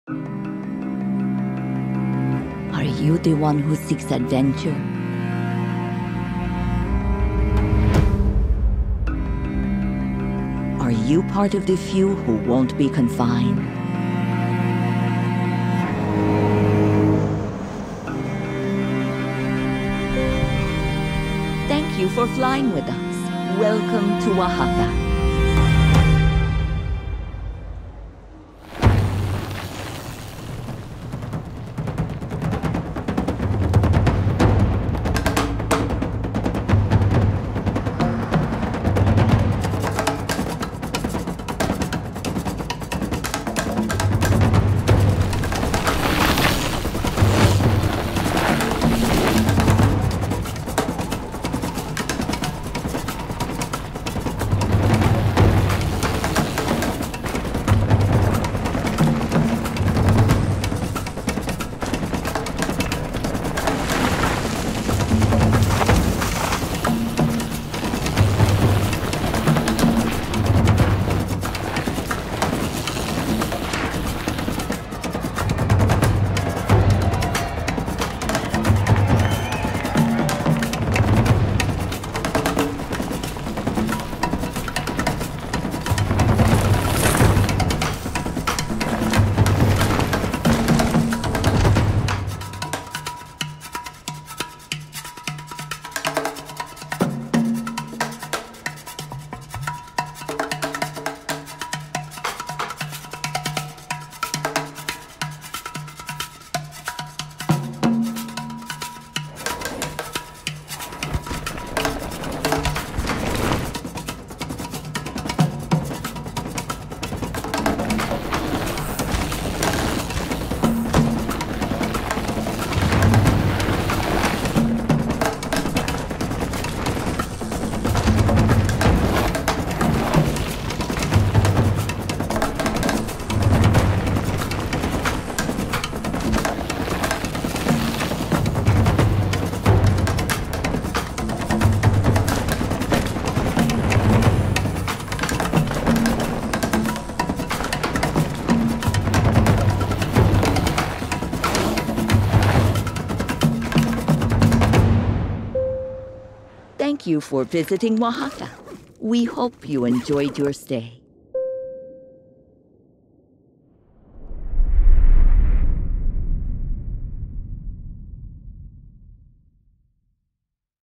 Escape to Oaxaca, Mexico
February in the north finds James Doerfling and Kurt Sorge smack in the middle of a long, frigid winter. The jumps in Kurt’s front yard are hibernating under an unusually deep snowpack, and the motor boat ride across the water from his home over to Nelson is especially choppy and cold. Meanwhile, never ending sub-freezing temperatures in Williams Lake, have James doubled up on long-johns and steadily idling on hot coffee. Diesels don’t like starting up in the cold, neither does the Goat.